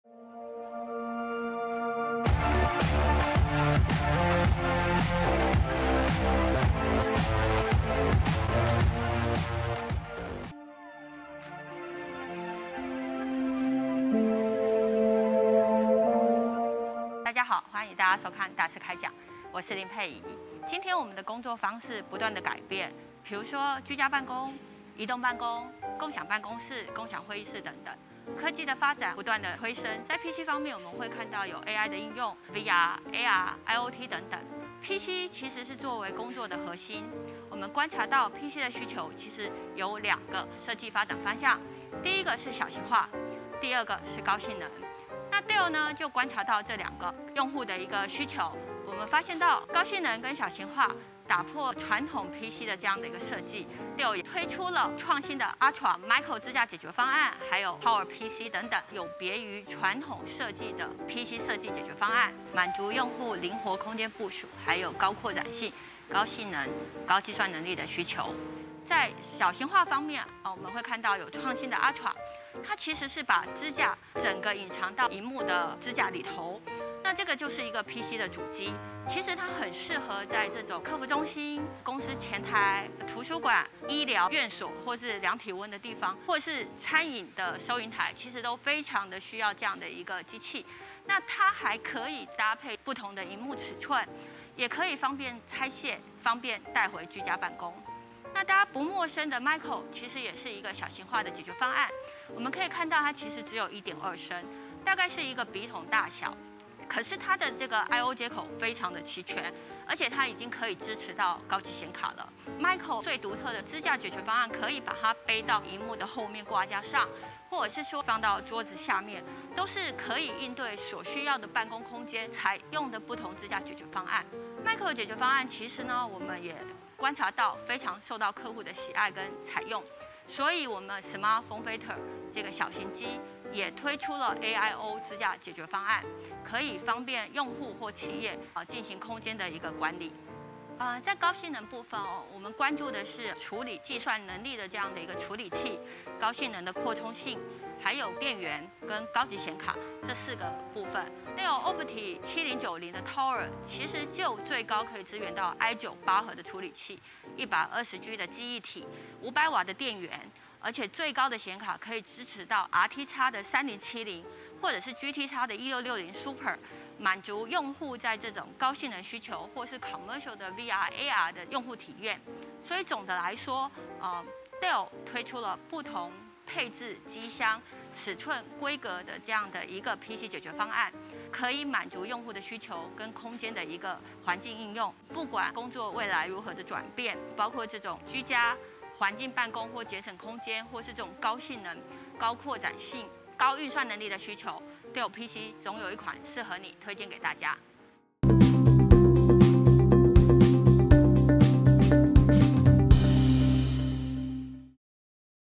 【 大師開講 EP87 】OptiPlex桌上型電腦
👩‍💻 大師開講系列 -- OptiPlex桌上型電腦

🎖主講人：
　 戴爾科技集團品牌經理 林佩儀

現代的工作環境除了疫情的影響不斷的改變
隨著科技的進步，AI、VR/AR、IoT等需求也同步提升
作為工作核心的PC為了因應這些轉變
在設計上也往 小型化、高性能 兩大重點發展

Dell 推出的
不僅在性能上可以滿足客戶需求
其中的支架解決方案更能發揮小型化節省空間的特色
想瞭解更多高這款性能、高擴展性、高運算能力的PC嗎？
千萬別錯過今天的大師開講～